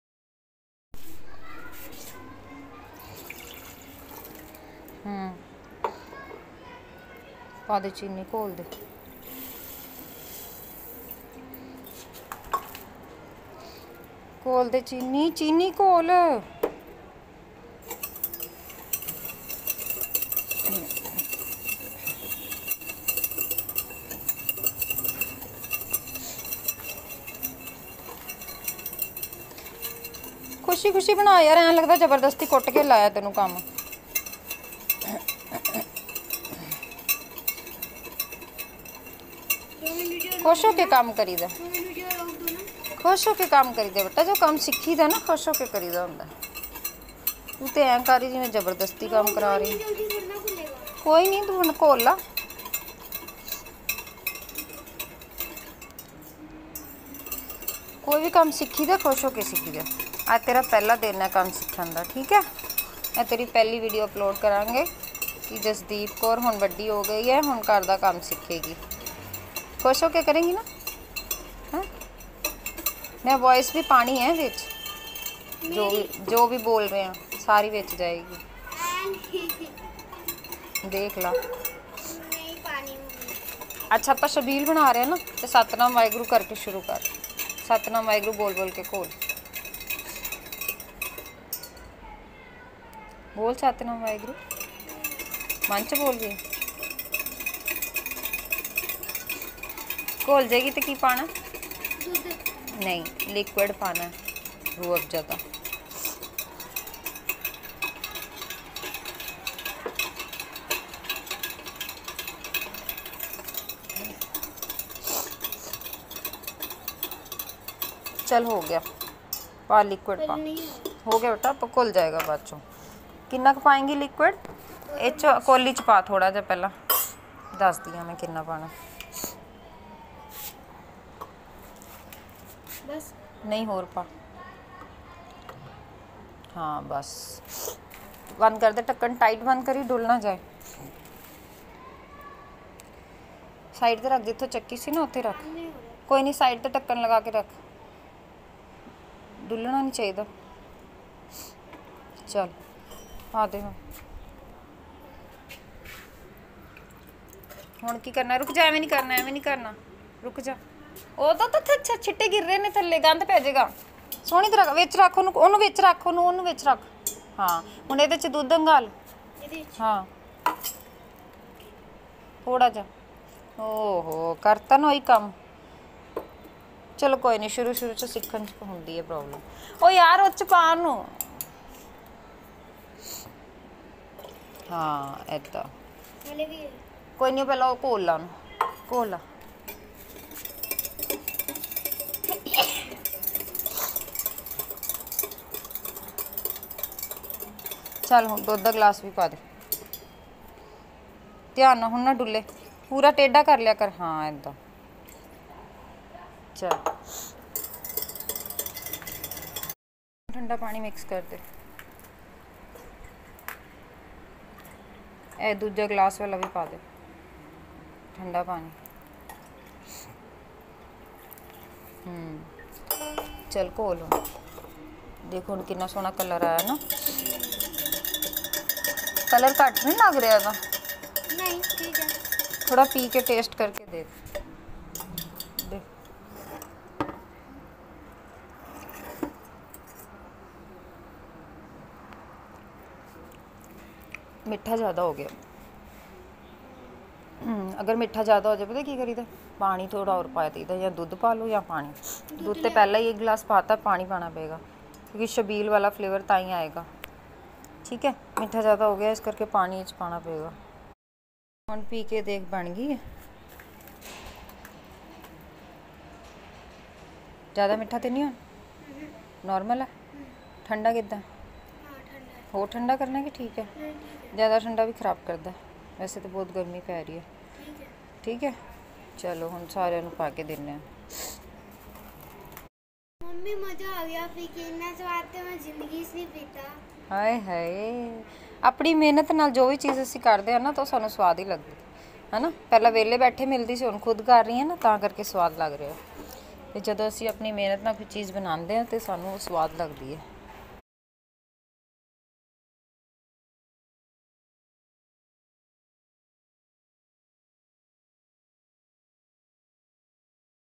ਸ਼ਬੀਲ ਕਿਵੇਂ ਬਣਾਈਏ, sharbat kaise banaen, शरबत कैसे बनाएं, Apne bacchon ko ghar ka kam kaise sikhayen
Apne bacchon ko ghar ka kam kaise sikhayen, Apne bacchon se Karen pyar se baat aur karaen ghar ka kam, bacchon Ko Bus jarurat hai pyar ki aur kuchh nahin, agar aapko kabhi Kam karana chahte Hain Apne bacchon se to pyar se bolo hamesha pyar se baat Karenਸ਼ਬੀਲ ਕਿਵੇਂ ਬਣਾਈਏ, sharbat kaise banaen, शरबत कैसे बनाएं